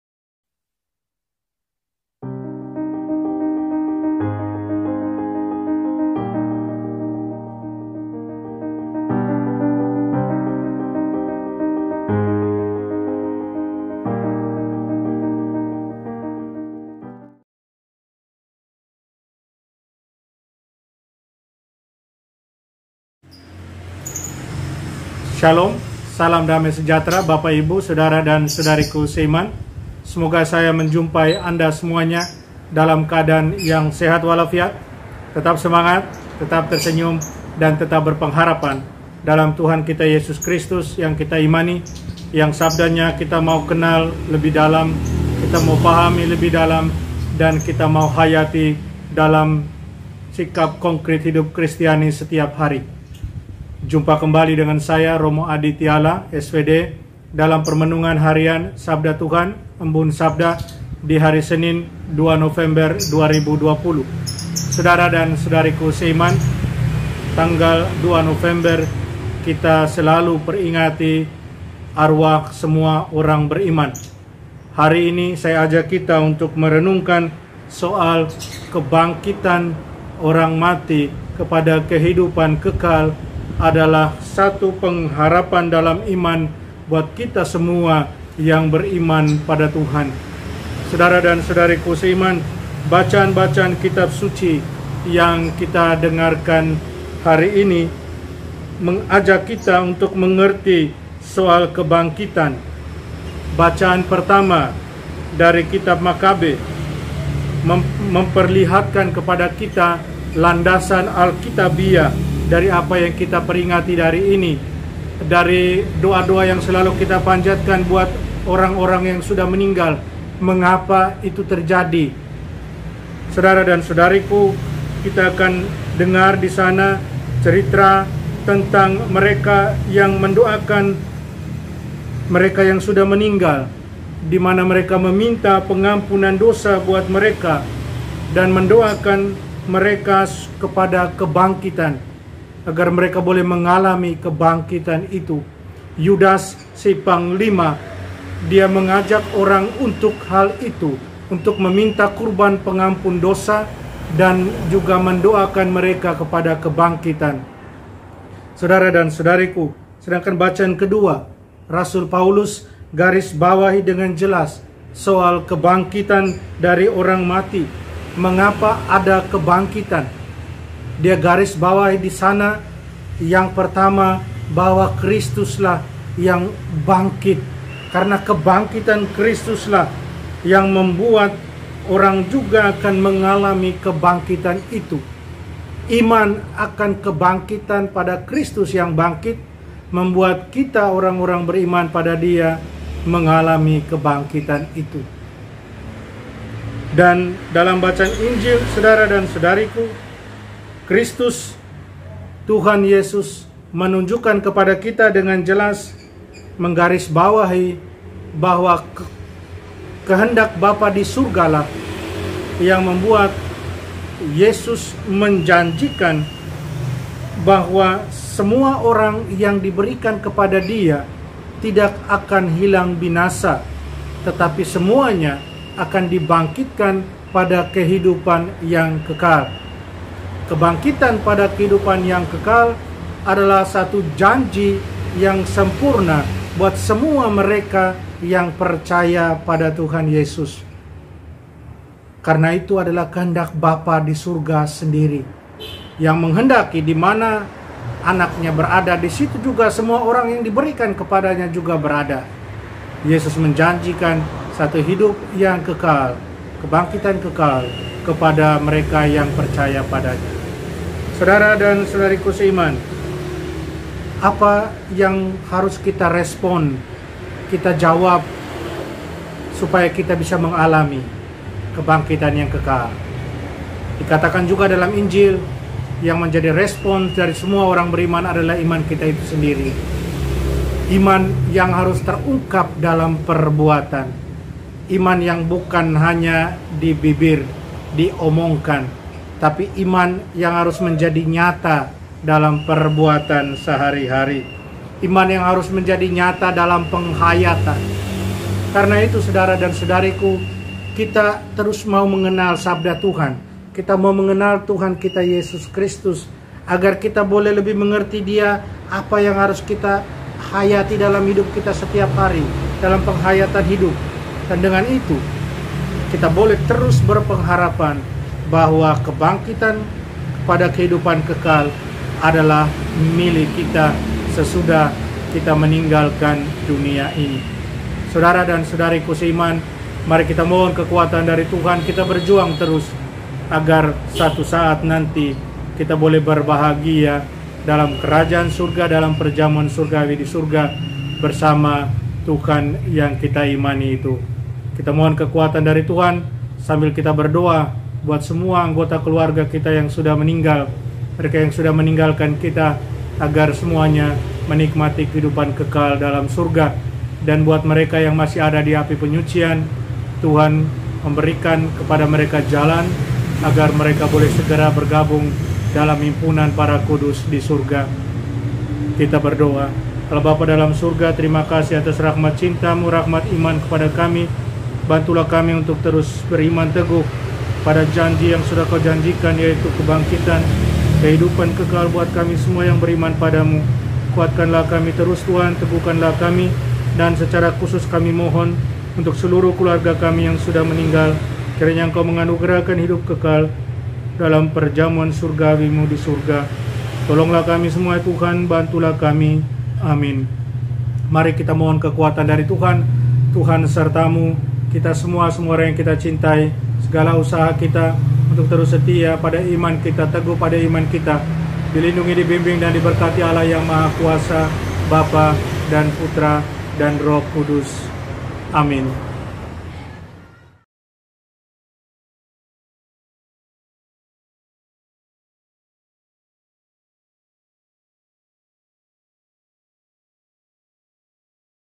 RenunganHarianKatolikEmbunSabda || Kebangkitan Orang Mati Pada Kehidupan Kekal - Pengharapan Iman
*Bacaan Liturgi 02 November 2020*

*Pengenangan Arwah Semua Orang Beriman*

*Bacaan Pertama*
*2Mak 12:43-46*

*Bacaan Kedua*
*1Kor 15:12-34*

*Bacaan Injil*
*Yoh 6:37-40*
Setiap orang yang percaya kepada Anak Allah
beroleh hidup yang kekal,
dan Tuhan akan membangkitkannya pada akhir zaman.
Inilah Injil Yesus Kristus menurut Yohanes:
Dalam rumah ibadat di Kapernaum
Yesus berkata kepada orang banyak,
"Semua yang diberikan Bapa kepada-Ku akan datang kepada-Ku,
dan barangsiapa datang kepada-Ku, ia tidak akan Kubuang.
Sebab Aku telah turun dari surga
bukan untuk melakukan kehendak-Ku,
tetapi untuk melakukan kehendak Dia
yang telah mengutus Aku.
Dan Inilah kehendak Dia yang telah mengutus Aku,
yaitu supaya dari semua yang telah diberikan-Nya kepada-Ku
jangan ada yang hilang,
tetapi supaya Kubangkitkan pada akhir zaman.
Sebab inilah kehendak Bapa-Ku,
yaitu supaya setiap orang,
yang melihat Anak dan yang percaya kepada-Nya
dan supaya Aku membangkitkannya pada akhir zaman."

Demikianlah Injil Tuhan!

*EMBUN SABDA:*
*"KEBANGKITAN ORANG MATI PADA KEHIDUPAN KEKAL - PENGHARAPAN DALAM IMAN."*

Saudara dan saudariku dalam Tuhan Yesus, jumpa kembali dengan permenungan harian sabda Tuhan, "Embun Sabda", edisi Senin, 02 November 2020. Hari ini dihari peringatan arwah orang orang beriman, saya ajak kita untuk merenungkan soal kebangkitan orang mati pada kehidupan kekal adalah pengharapan dalam iman dari semua yang beriman pada Tuhan.
Saudara dan saudariku, bacaan bacaan Kitab Suci hari ini sungguh mengajak  kita untuk mengerti soal kebangkitan pada kehidupan kekal yaitu kita imani dalam iman akan Yesus Kristus.
Bacaan pertama dari Kitab Makabe memperlihatkan landasan kitab suci dari apa yang kita peringati hari ini. Kita mendoakan mereka yang sudah meninggal meminta pengampunan dosa buat mereka, dan mendoakan kebangkitan mereka.
Sedangkan bacaan kedua, Rasul Paulus garisbawahi dengan jelas soal kebangkitan dari orang mati dikarenakan oleh Kristus sendiri yang telah bangkit, dan karena iman akan Yesus yang telah bangkit. Dan ini adalah pengharapan kita dalam iman akan Yesus Kristus. Kristus adalah buah sulung dalam kebangkitan. Semua yang sudah meninggal, dihidupkan kembali dalam persekutuan dengan Kristus.
Saudara dan saudariku, sedangkan dalam Injil, Tuhan Yesus garis bawahi soal kehendak Bapa Disurga yang menghendaki agar semua orang yang diberikan kepada Yesus tidak ada yang hilang tetapi dibangkitkan pada akhir jaman dan memperoleh hidup kekal, dan bahwa supaya itu menjadi kenyataan setiap orang harus meresponnya dengan iman akan Yesus, iman akan kebangkitan kekal, iman akan kehidupan kekal.
Saudara dan saudariku, mari kita teguhkan iman kita akan kebangkitan, akan kehidupan kekal, dan oleh karena itu kita perdalam penghayatan kita akan iman itu lewat sikap hidup kristiani setiap hati.
Saudara dan saudariku, mari kita terus berjuang untuk kehidupan kekal dan selalu hidup dalam pengharapan. Amin. Semoga.

Allah Bapa dalam Surga, terima kasih dan syukur atas segala kasih karuniaMu. Bantulah kami teguh dalam iman, dan bantulah kami agar selalu hidup dengan iman kita itu agar kita beroleh kasih karunia kita, yaitu Kebangkitan dan Kehidupan Kekal. Amin

Tuhan Yesus menyertai dan memberkati kita semua (Dan Keluarga) dan seluruh kehidupan kita

*RP. Lukas Gewa Tiala (Adi), SVD*